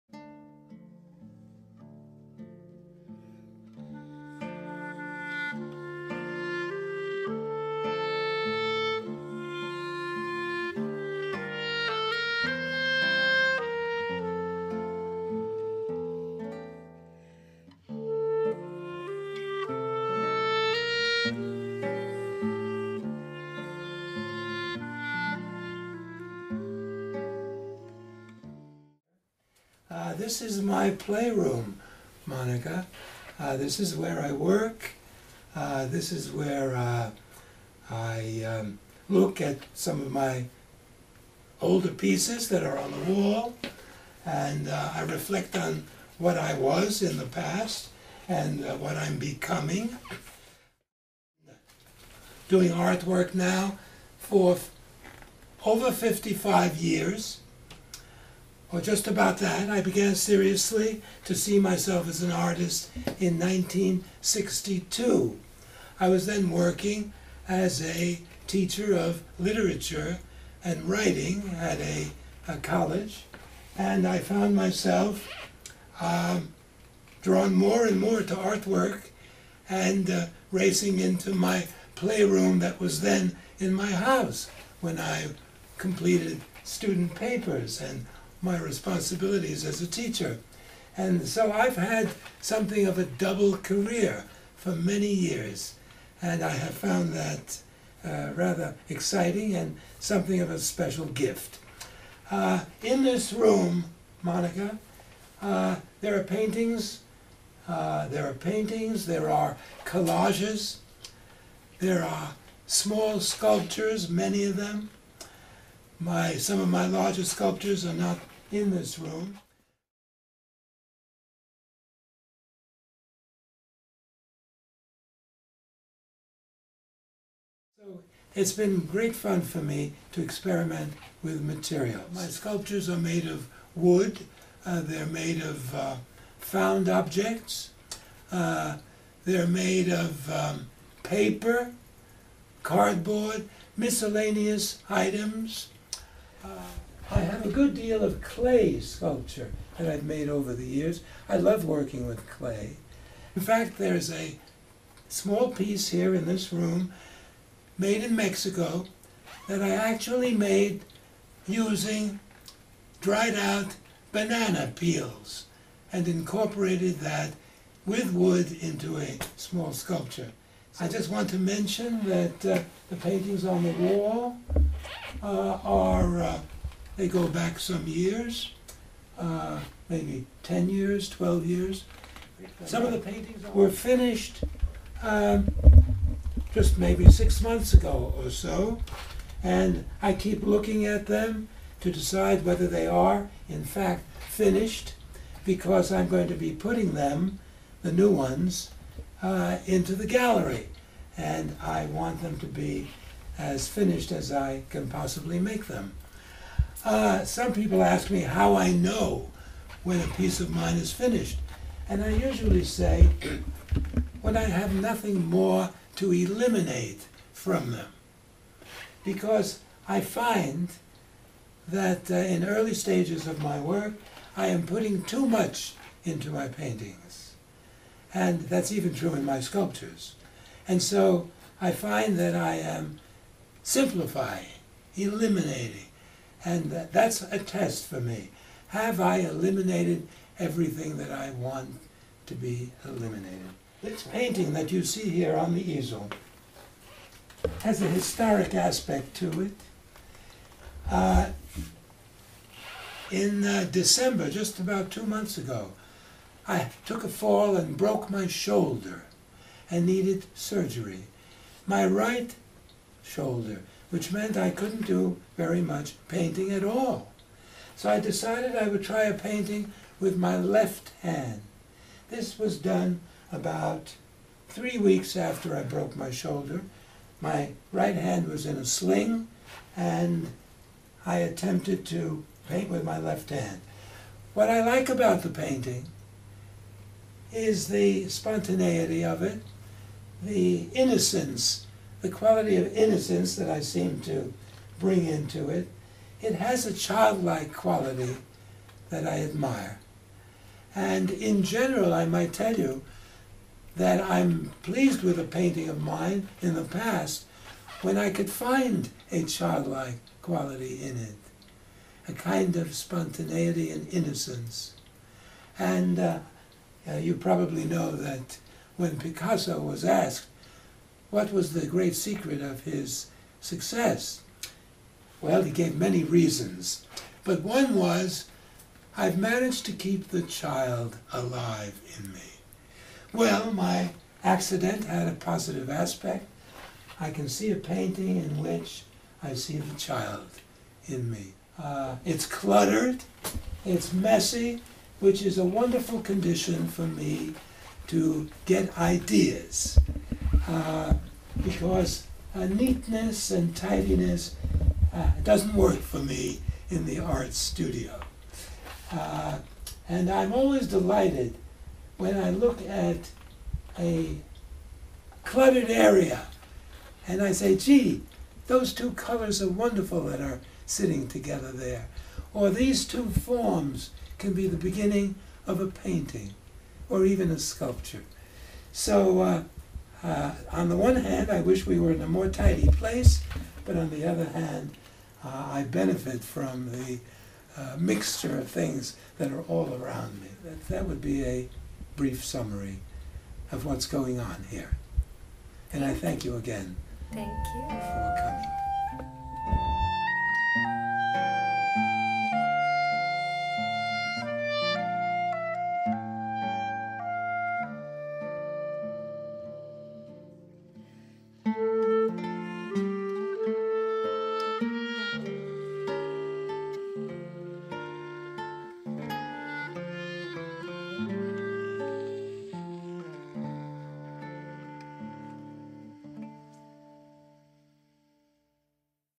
Brewster Cultural Council Presents 12th Annual Art Show
2018 Art Exhibit Honoree Milton Teichman talking about his work (videographed & edited by Monika Woods, music by John Dirac guitar, and Monika Woods clarinet)